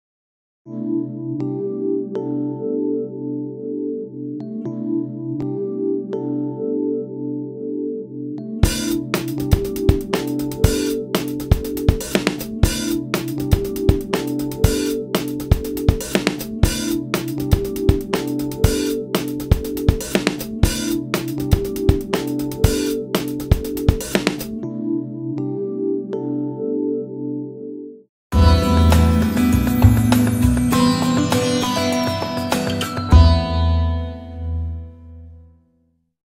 Funk Drive
28 second experimental art film: Funk Drive. A psychedelic colour scheme with funky beats, and a precision driver behind the wheel.